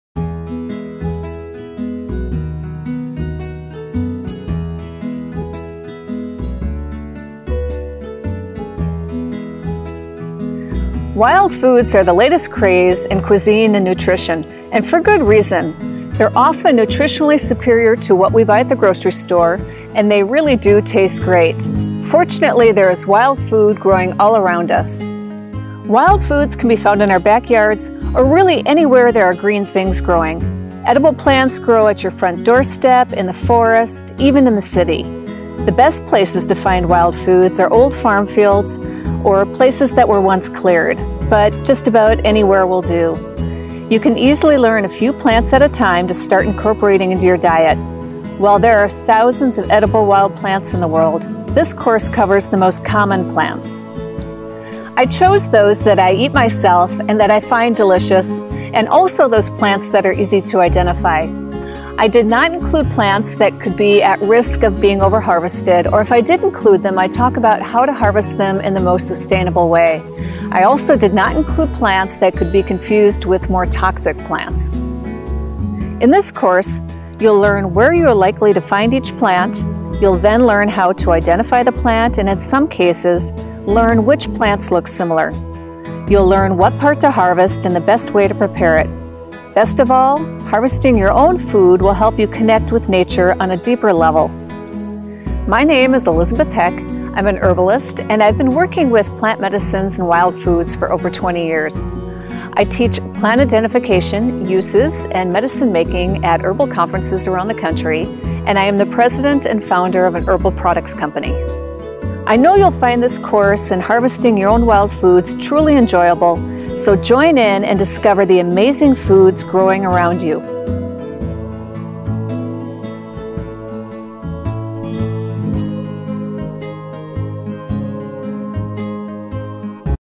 Welome to Wild Foods: Identifying, Harvesting and Preparing
Wild Foods :: Identify, Harvest and Prepare Edible Plants
Learn how to identify wild foods as well as forage for and prepare them. We'll cover the most amazing wild edible plants